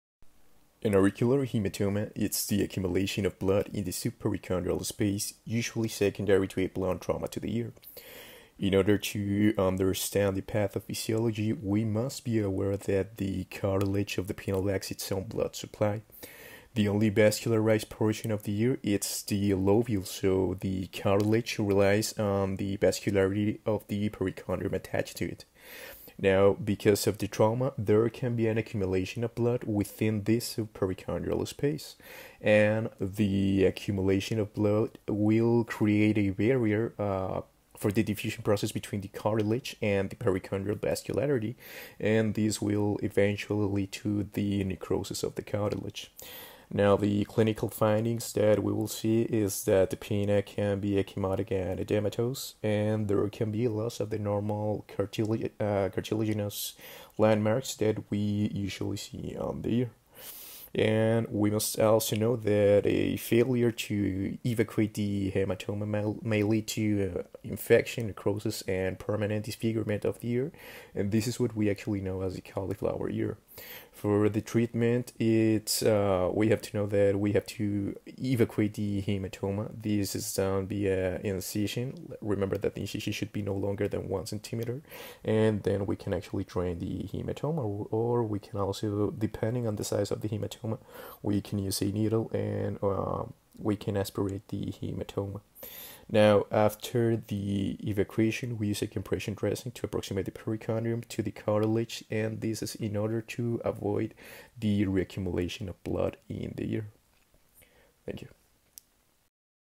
CAULIFLOWER EAR - AURICULAR HEMATOMA
Find out what an auricular hematoma ear, why it can progress to a cauliflower ear, diagnosis and treatment.

Cauliflower ear in fighters.